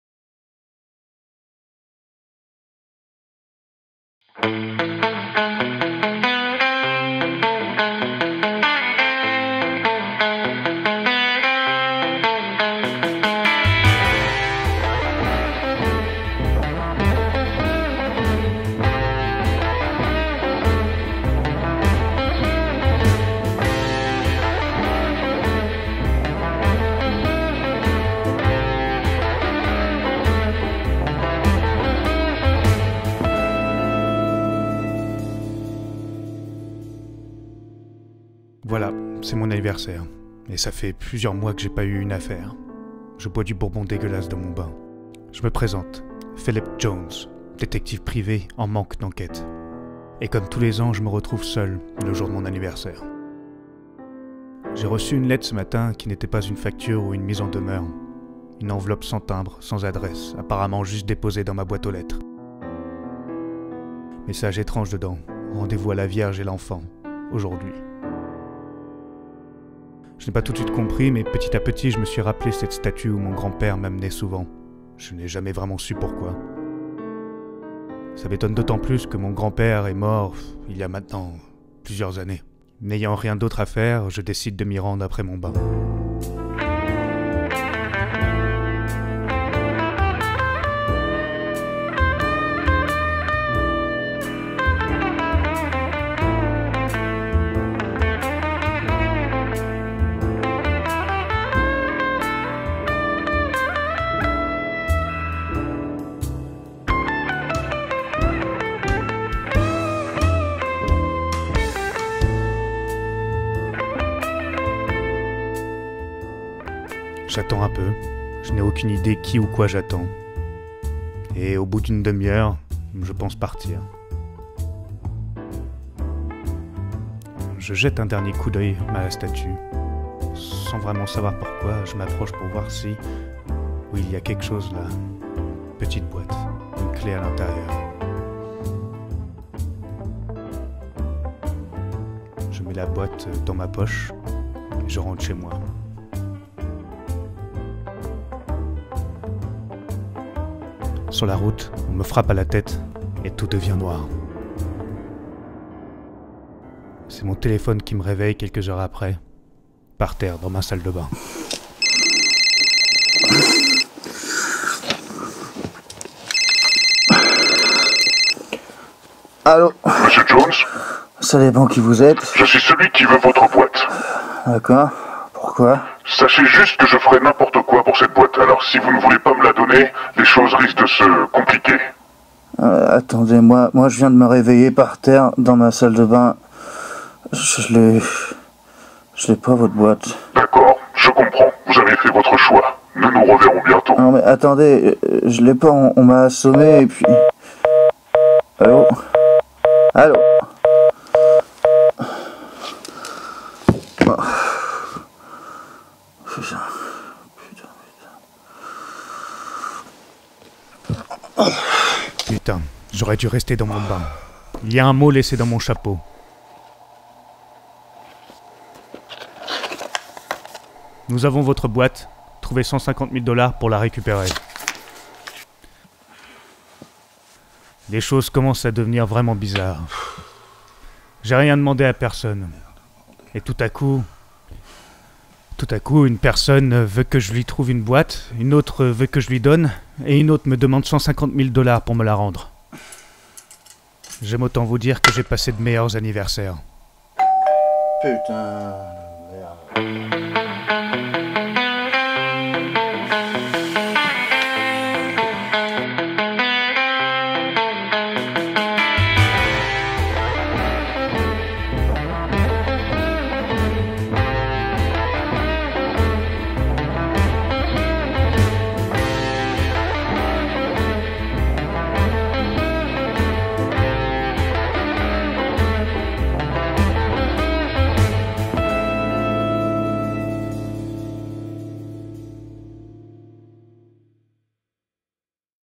Un Film Noir - Episode 1 - T.R.Josset's Shorts
Premier épisode d'une série. L'histoire de Philip Jones, Détective privé à qui il arrive plein de choses qu'il ne contrôle pas.